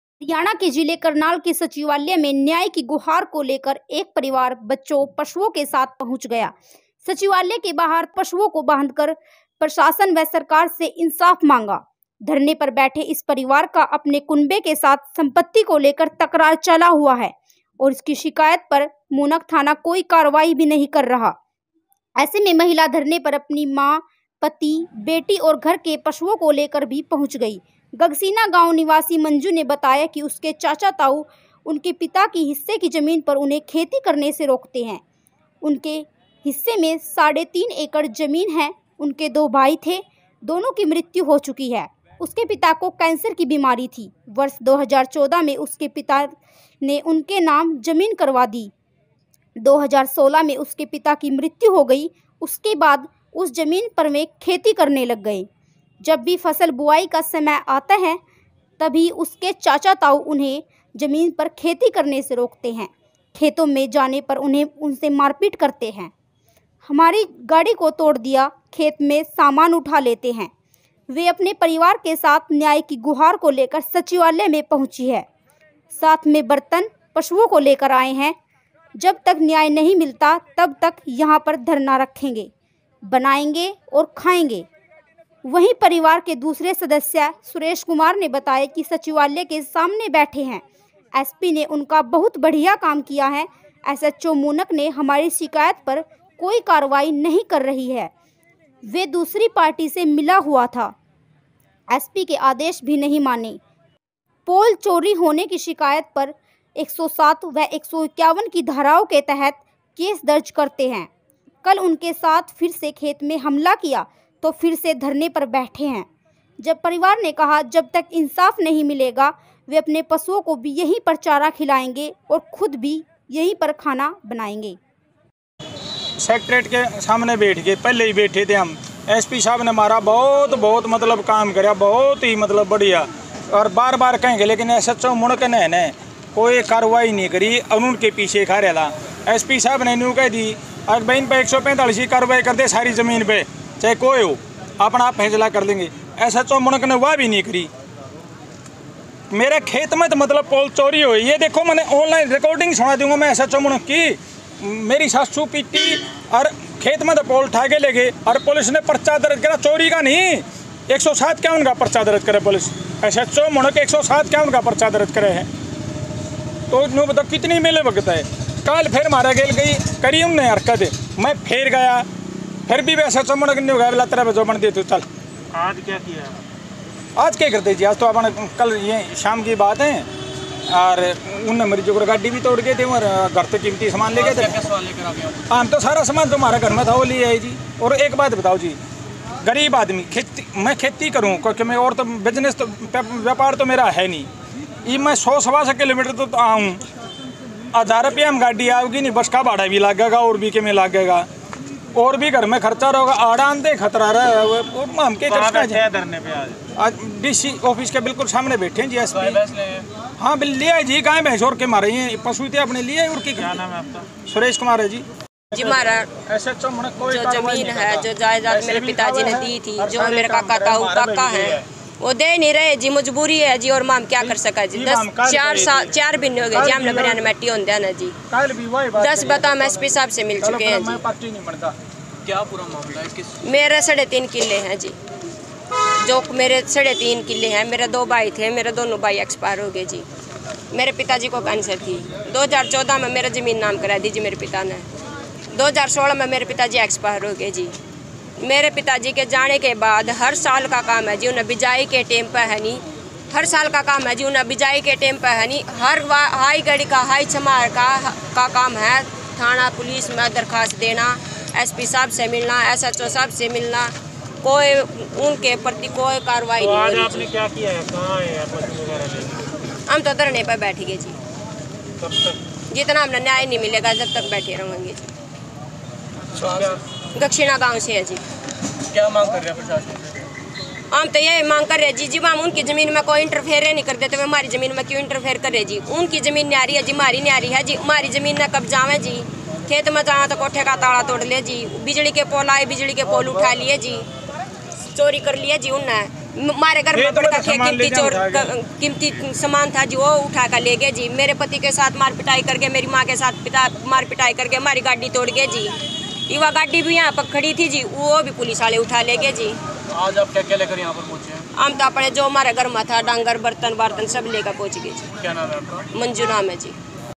करनाल के सचिवालय में न्याय की गुहार को लेकर एक परिवार बच्चों-पशुओं के साथ पहुंच गया।
हरियाणा के जिले करनाल के सचिवालय में न्याय की गुहार को लेकर एक परिवार बच्चों-पशुओं के साथ पहुंच गया। सचिवालय के बाहर तीनों पशुओं को बांधकर प्रशासन व सरकार से इंसाफा मांगा। धरने पर बैठे इस परिवार का अपने कुनबे के साथ संपत्ति को लेकर तकरार चला हुआ है और इनकी शिकायत पर मूनक थाना कोई कार्रवाई भी नहीं कर रहा। ऐसे में महिला धरने पर अपनी मां, पति, बेटी और घर के पशुओं को भी लेकर पहुंच गई। गगसीना गांव निवासी मंजू ने बताया कि उसके चाचा-ताऊ उसके पिता के हिस्से की जमीन पर उन्हें खेती करने से राेकते हैं। उसके हिस्से में साढ़े तीन एकड़ जमीन है। उसके दो भाई थे, दोनों की मौत हो चुकी है। उसके पिता का कैंसर की बीमारी थी। वर्ष 2014 में उसके पिता ने उसके नाम जमीन करवा दी। 2016 में उसके पिता की मौत हो गई। इसके बाद उस जमीन पर वे खेती करने लग गए। जब भी फसल बुआई का समय होता है तभी उसके चाचा-ताऊ उन्हें जमीन पर खेती करने से रोकते हैं। खेतों में जाने पर मारपीट करते हैं। हमारी गाड़ी को तोड़ दिया। खेत से सामान उठा लेते हैं। वे अपने परिवार के साथ न्याय की गुहार लेकर सचिवालय में पहुंची है। साथ में बर्तन, पशुओं को लेकर आए हैं। जब तक न्याय नहीं मिलता तब तक यहां पर धरना रखेंगे बनाएंगे और खाएंगे। वहीं परिवार के दूसरे सदस्य सुरेश कुमार ने बताया कि सचिवालय के सामने बैठें है। एसपी ने उनका बहुत बढ़िया काम किया। एसएचओ मूनक ने हमारी शिकायत पर कोई कार्रवाई नहीं कर रही है। वे दूसरी पार्टी से मिला हुआ था। एसपी के आदेश भी नहीं माने। पोल चोरी होने की शिकायत पर 107 व 151 की धाराओं के तहत केस दर्ज करते हैं। कल उनके साथ फिर खेत में हमला किया तो फिर से धरने पर बैठे हैं। परिवार ने कहा कि जब तक इंसाफ नहीं मिलेगा अपने पशुओं को भी यहीं चारा खिलाएंगे और खुद भी यहीं खाना बनाएंगे।